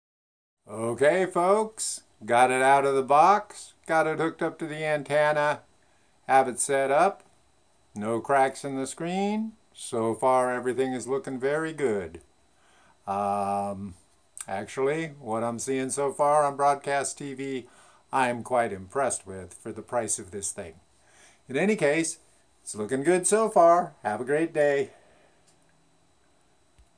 Unboxing of Sceptre U550CV-U 55" 4K Ultra HDTV - Part 3
The Sceptre U550CV-U 55" 4K Ultra HD TV is the bargain basement King of 4K TVs with a huge screen and a price under $400 at Wal mart. Mine just arrived and here is what the unboxing looks like. More to come.